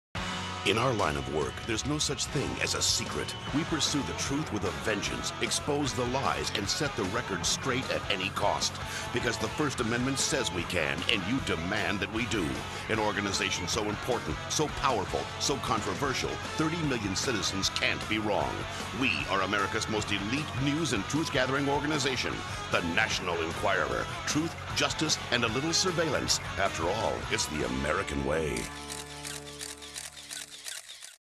National Enquirer
This National Enquirer :30 TV spot - "A Little Surveillance" spoofs "investigative journalism" and features reporters posing as secret agents. Written, produced, directed and edited by Terry Moloney.